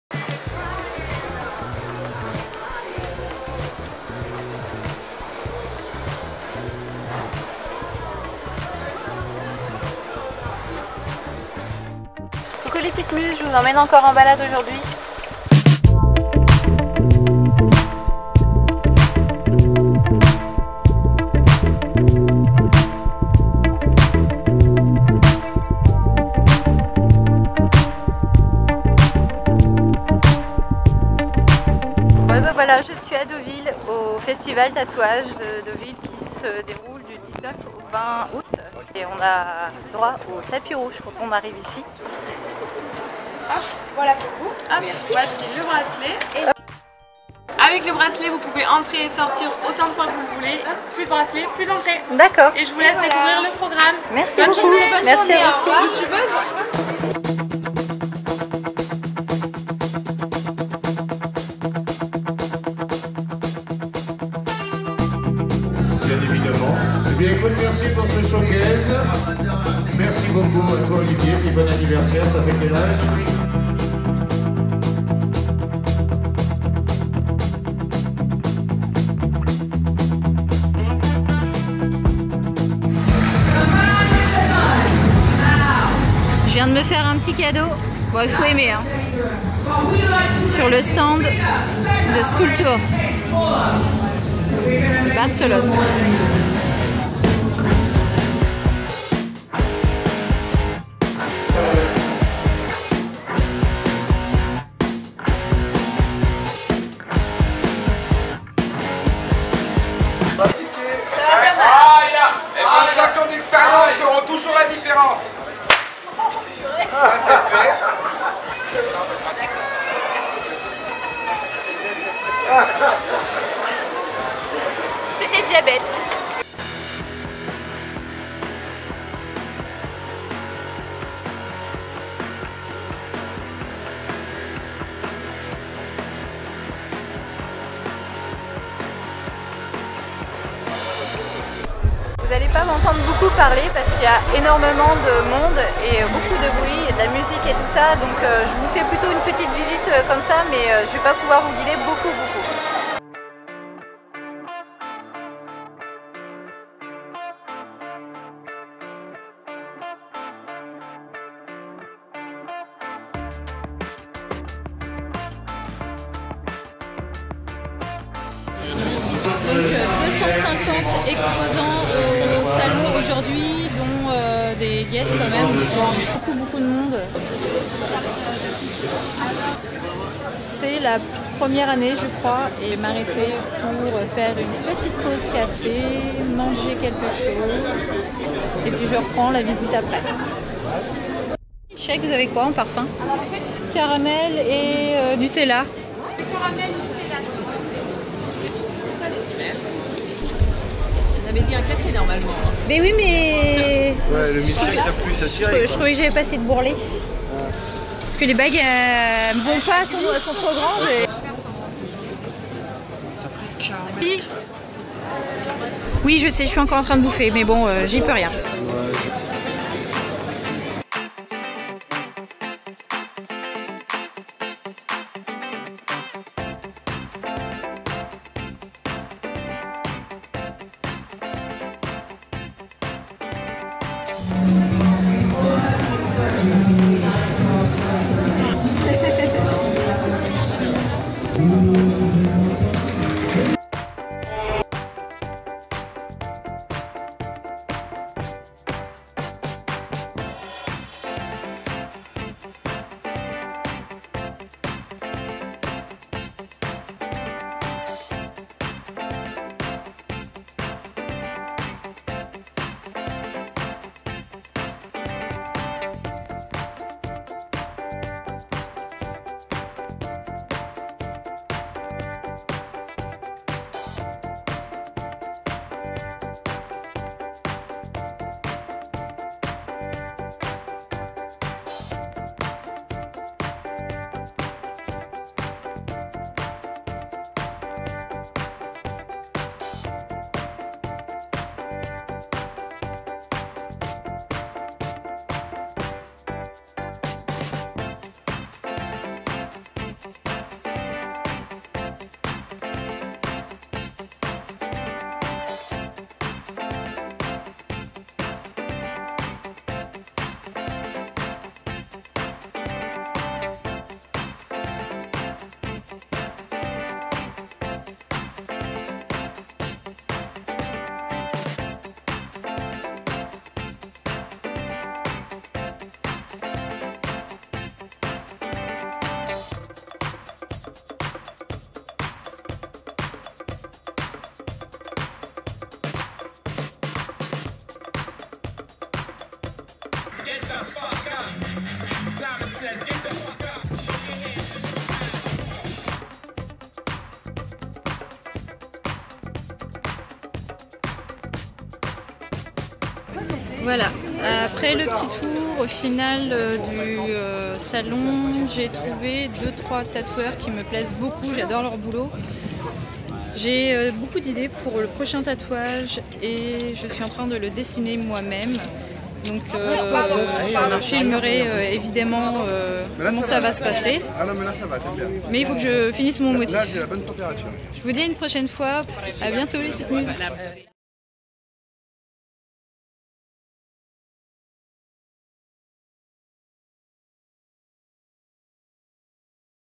Deauville tattoo festival cid ☠️ VLOG
Hello les p'tites Muses !! Alors ! cette visite à deauville ???
Je suis une tattoo maniaque. J'ai dessiné mon prochain tatouage et je suis à la recherche de mon futur tatoueur. Je suis donc allée au festival tattoo convention sur Deauville.

Pour discuter et en voir davantage en photo et en vidéo venez sur mon Vlog

Mon Instagram : @steph_bodypainter

Merci de m'avoir accompagné pour cette balade.